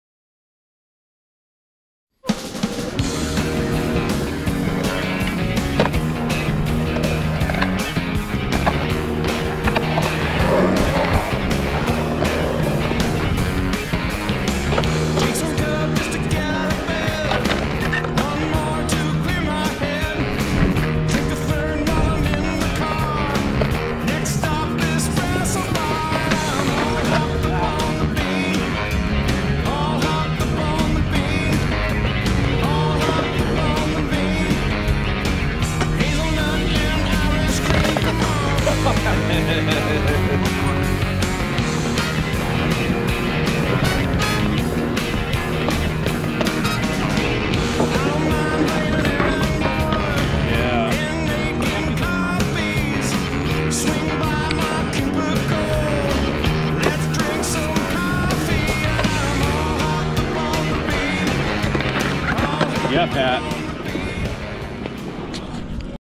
Feeble Grind Transfer Over The Spine!!
Slane18 productions teams up with lil Pat Calwell to film a Sk8 part at the Augusta Skatepark in Maine.